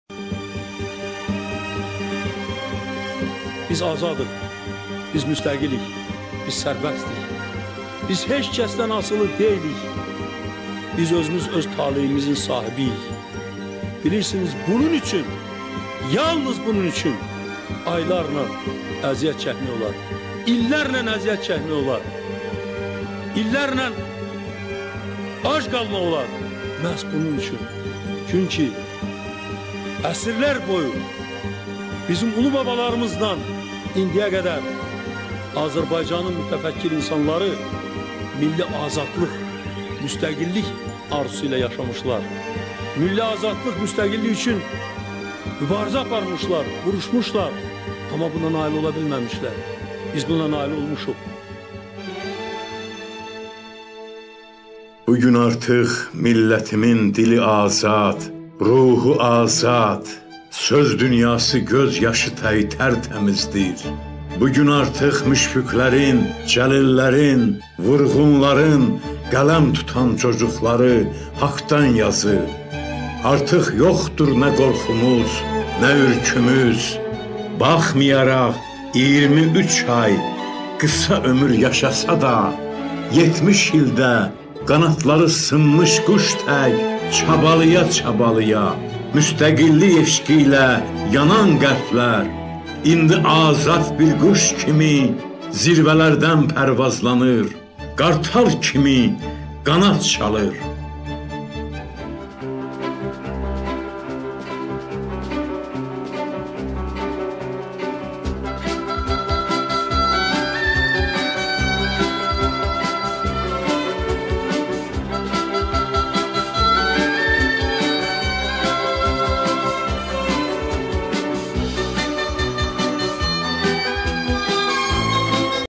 Tərtər rayon MKS-nin Soyulan kənd kitabxana filialı 28 may Respublika Günü aid videoçarx hazırlayıb
Tərtər rayon MKS-nin  Soyulan   kənd  kitabxana filialı   28 may Respublika Günü  ilə əlaqədar videoçarx hazırlayıb.